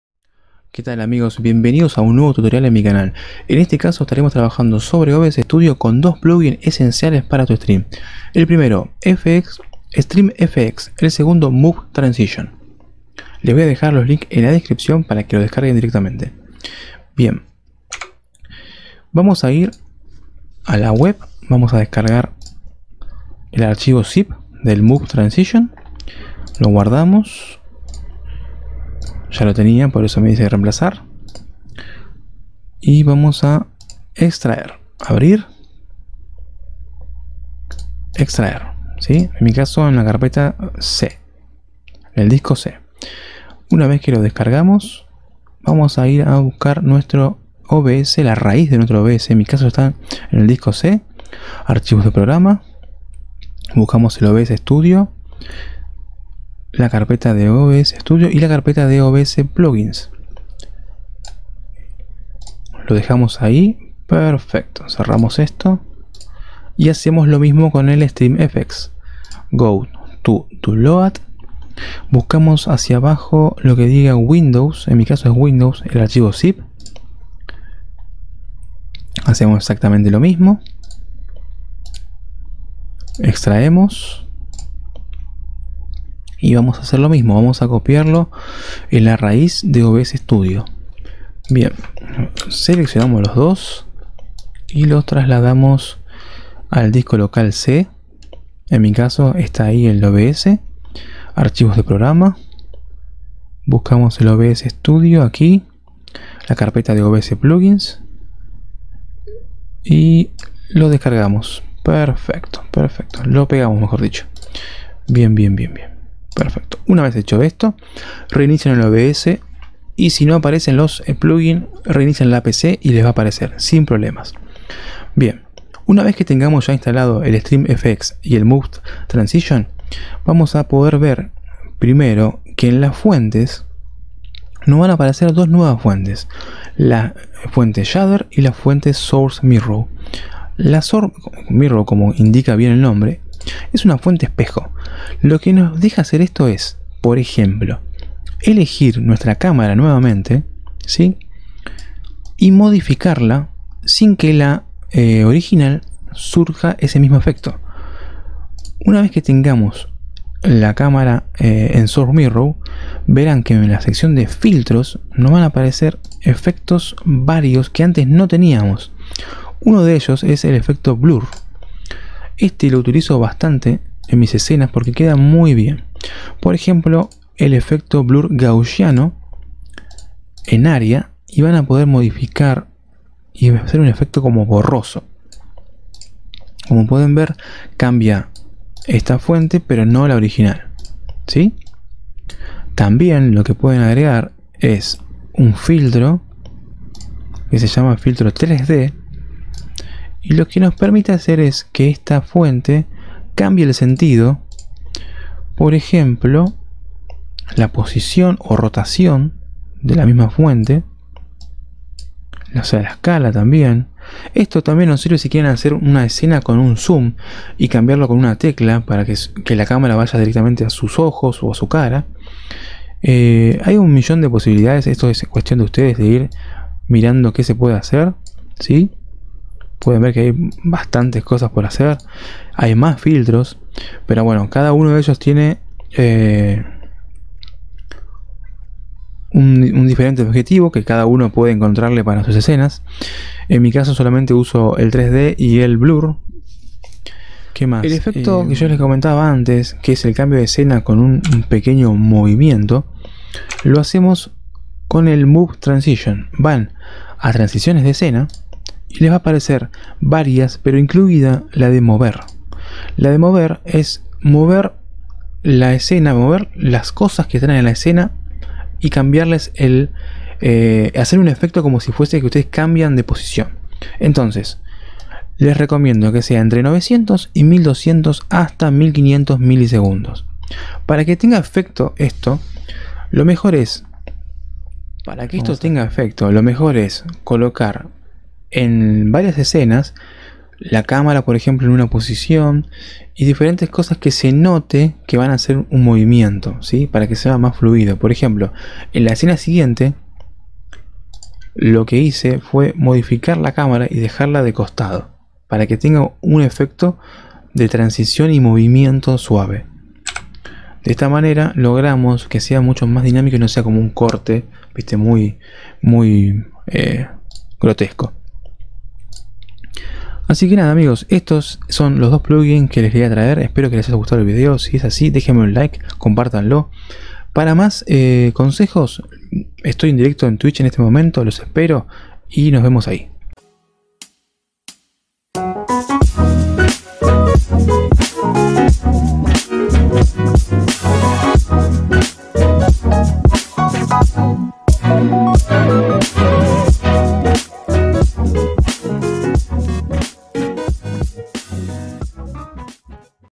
👌EFECTO BORROSO OBS STUDIO | STREAMFX MOVE TRANSITION 2023
STREAMFX MOVE TRANSITION 2023
♋Seguimos con la serie ayudando streamers en este caso les traigo un mini tutorial de como instalar dos plugins es obs studio el stream fx y el move transition que los ayudara a crear escenas mas originales.